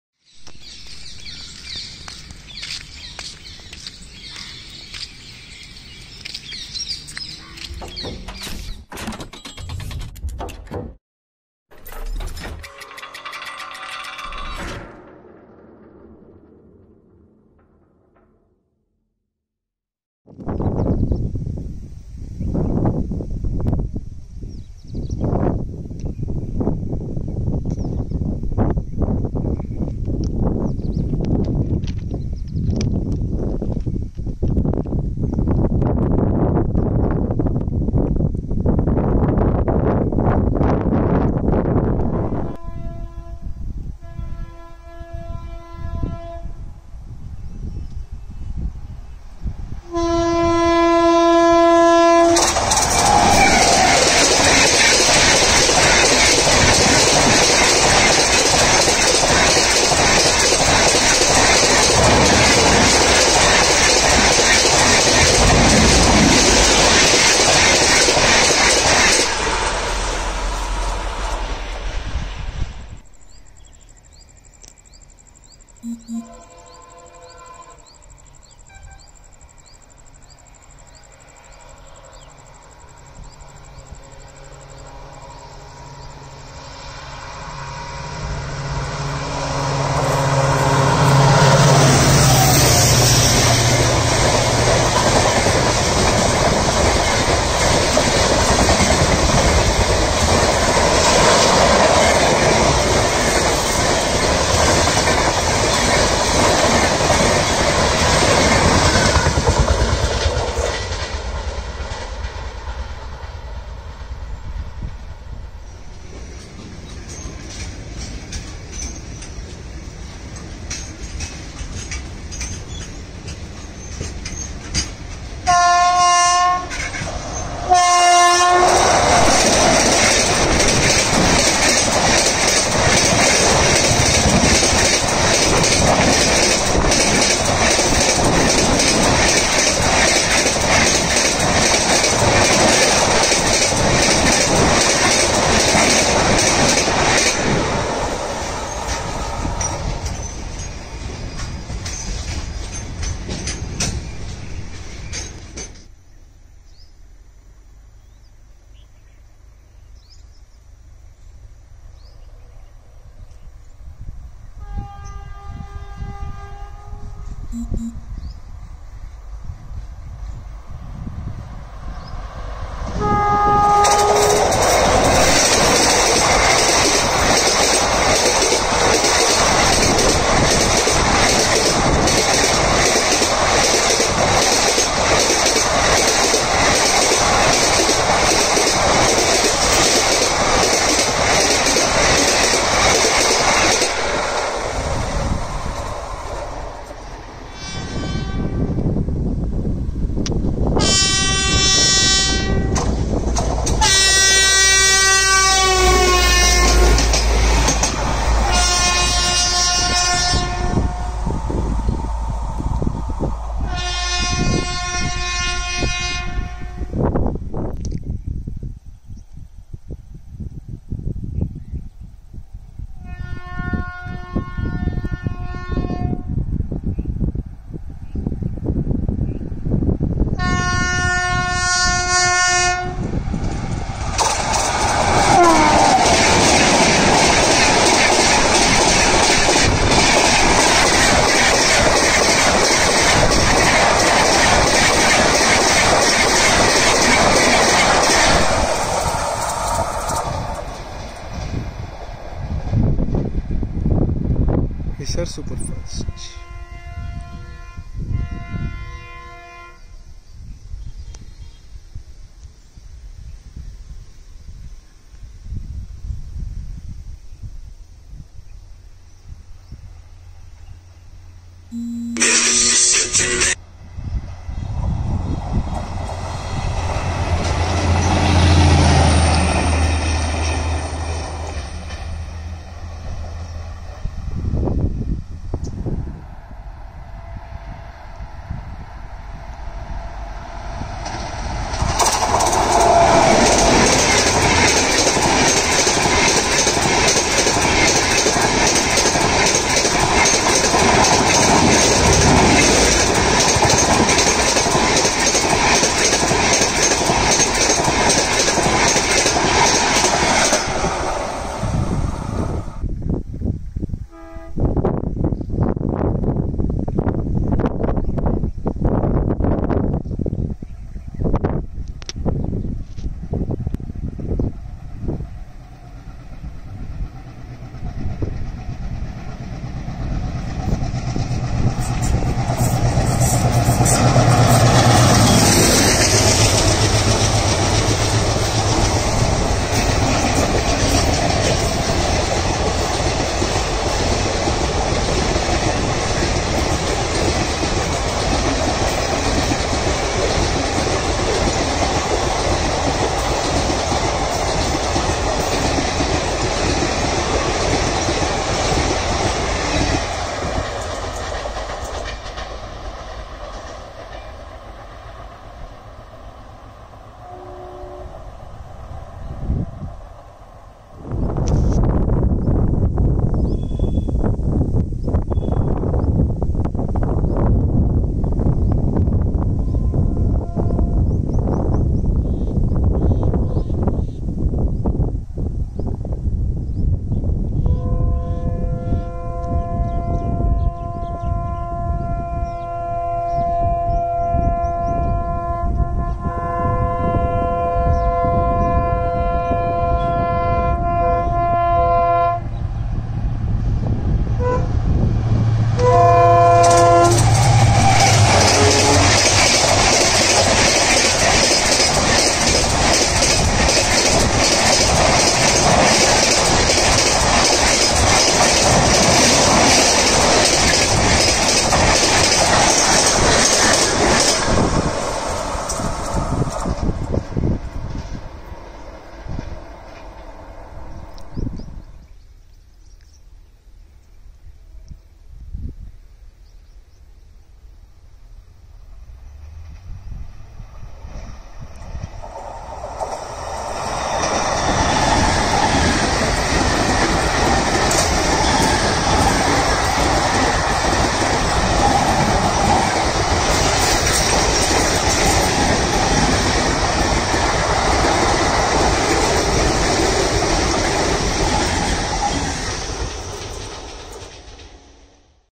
130 Kmph High Speed Diesel & Electric Hauled Trains With Lovely Track Sounds near Vadodara
3 Diesel Hauled Trains

1. Ajmer - Bengaluru Express hauled by UBL WDP4 at 105 Kmph
2. Okha - Ernakulam Express hauled by ERS WDM3A at 110 Kmph
3. Bengaluru - Jodhpur Express hauled by KJM WDP4B at 125 Kmph

4 Electric hauled Trains

1. Bandra - Vaishno Devi Katra Swaraj Express hauled by GZB WAP7
2. Bandra - Ghazipur Clone Humsafar Express hauled by BRC WAP7
3. Mumbai Central - Samastipur Express hauled by ED WAP4
4. Bandra - Hisar Superfasr Express hauled by BRC WAP7
5. Bandra - Amritsar Paschim Express hauled by BRC WAP7